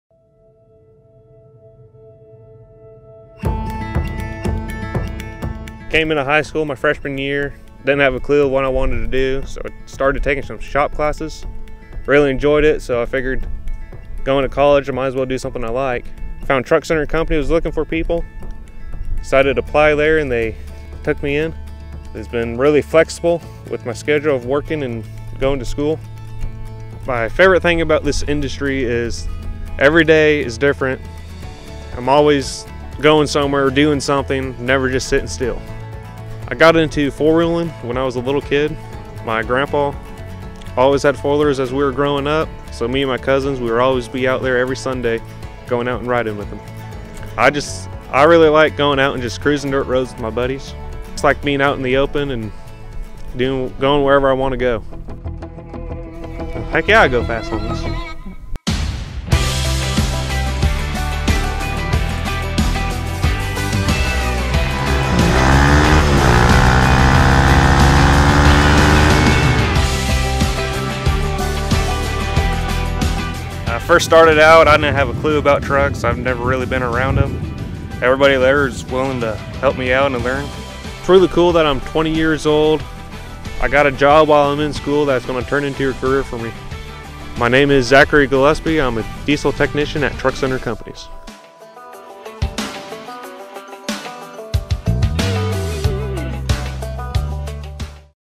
TCC - Diesel Tech Zackary
Get to know Zackary and how he found his career as a diesel technician for Truck Center Companies.